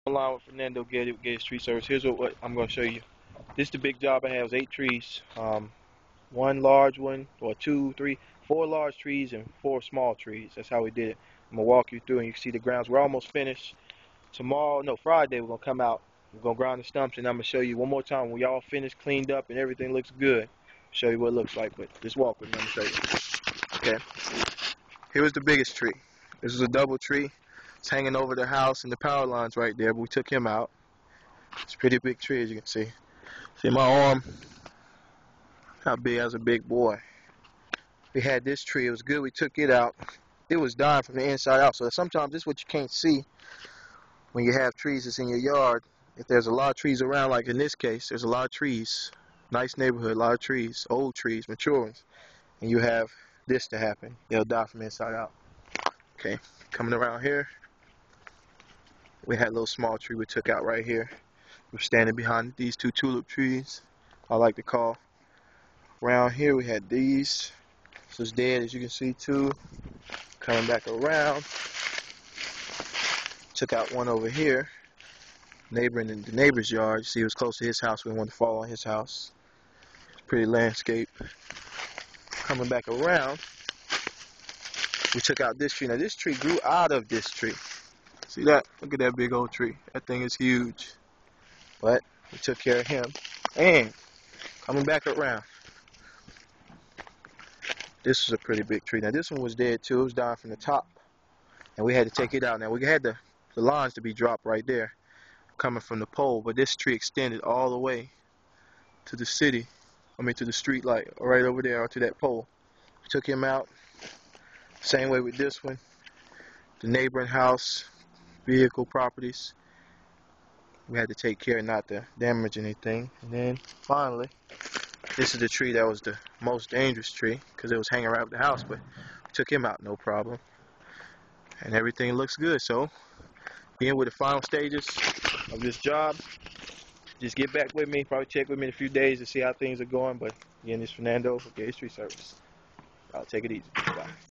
Mission 4.3: A Job Well Done - Geddie's Tree Service
My 'assessing the two popular trees' video shows the grounds and how they look now and they do indeed look great.  Another truly satisfied customer.  Mission 4 was a huge success.  Thx-4-Watching!